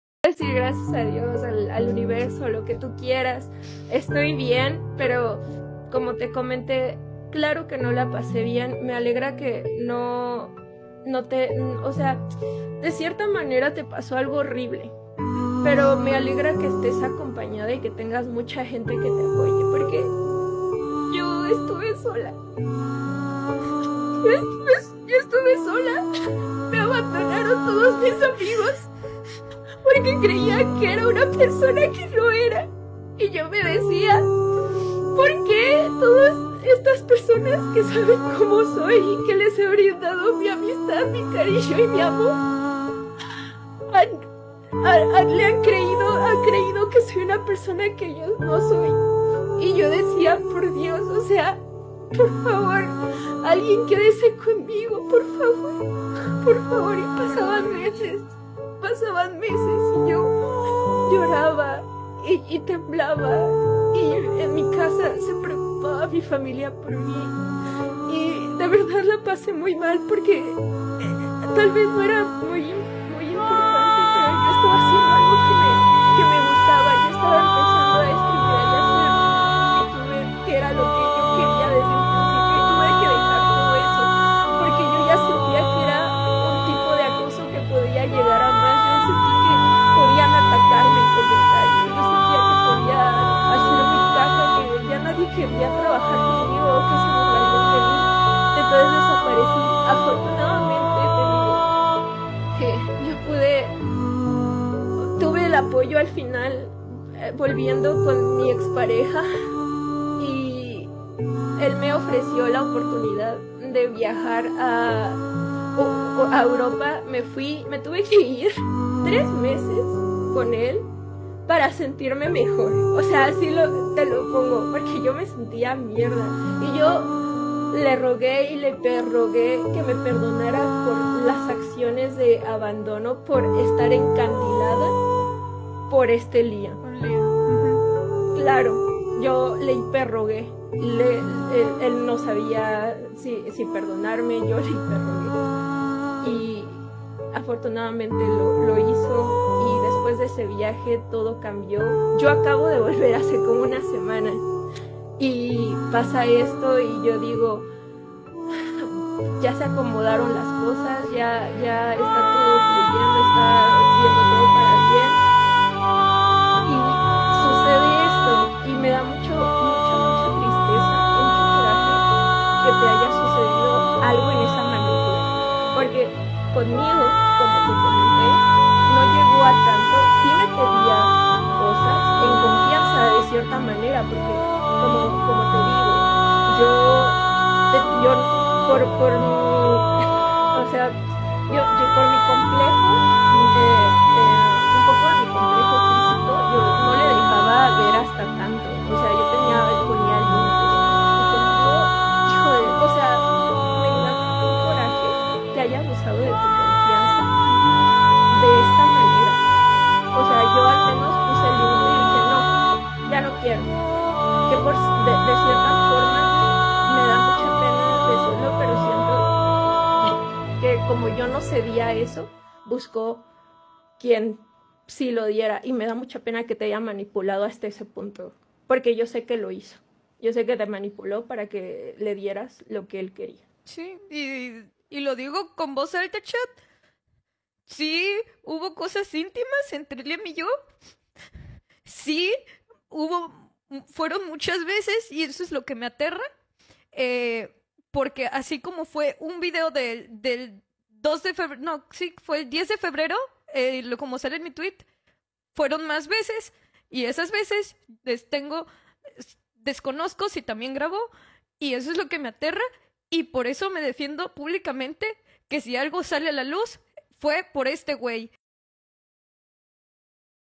CHERRYAHRIZONA LLORA CUENTA LO SUCEDIDO DIRECTO CON LIAM VIDEL POR VIDEO (GRABACIÓN DE EL SUCESO)
Hola espero les guste el video SIGANME SI QUIEREN JUGAR CONMIGO :3
ROBLOX: pollomapita21
SIGIUEME PARA MAS XD